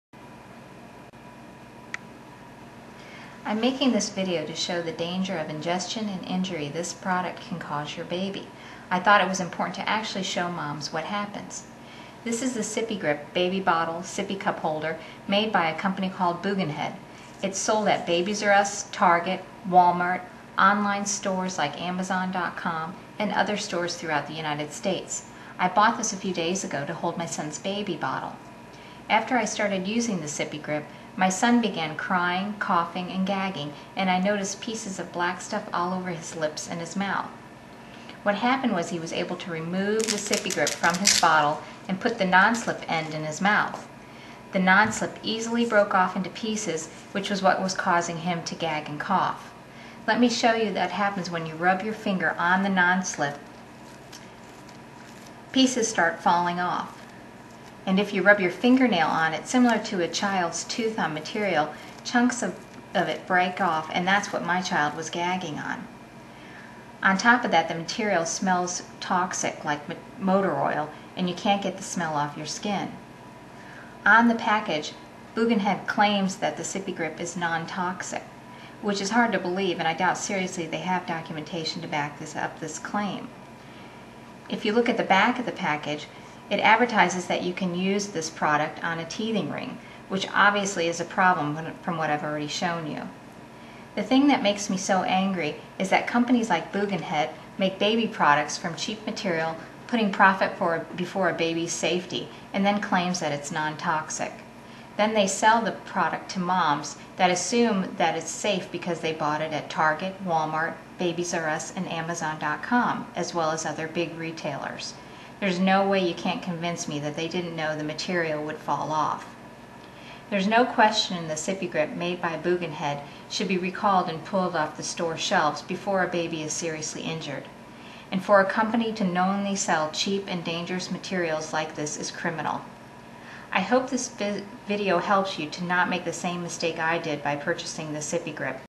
SippiGrip Should Be Recalled
I am making this review to show the danger of ingestion and injury that the SippiGrip can cause your baby. I made a video for this review because I thought it was important to actually show moms what happens.
The SippiGrip is sold at Babies R Us, Target, Wal-Mart, Amazon.com and other stores through out the U.S. I bought the SippiGrip to hold my sons bottle.
What happened was my son started crying, coughing and gagging and I notice pieces of black stuff all over is his lips and in his mouth. What happened was he was able to remove the SippiGrip from his bottle and put the non-slip end in his mouth. The non-slip easily broke off in pieces in his mouth and that is what was causing the coughing and gagging. It was difficult to flush out the pieces because they were sticking to the inside of his mouth.
If you rub your finger back and forth across the non-slip pieces easily start falling off, and if you rub your finger nail on it which would be similar to a childs tooth on the material, chunks break off and this is what my baby was gagging on and swallowed.
Now, on top of that the material really smells toxic like motor oil and you cant get smell off of your skin.
On the package Booginhead claims the SippiGrip is Non-Toxic which is impossible to believe and I doubt very seriously they have any documentation to back that up that claim.
The back of the package advertises that you can use it with a teething ring which is ridiculous when you see how easily the material breaks off and how a baby can swallow it.
The thing that makes me so angry is that companies like Booginhead make baby products like the SippiGrip from cheap material putting profit before a babys safety and make claims it is safe and non toxic. Then sell them to moms that assume the product is safe because they bought it a big retailer like Target, Wal-Mart, Babies R Us and Amazon. There is no way you can convince me they didnt know this material falls apart.
There is no question the SippiGrip by Booginhead should be recalled and pulled from the store shelves before a baby is seriously injured. And for a company to knowingly use cheap and dangerous materials like this is criminal.
I hope this review and video keep you from making the same mistake it did by purchasing the SippiGrip. I will certainly be looking at the product I buy for my baby a lot closer and I hope you will as well.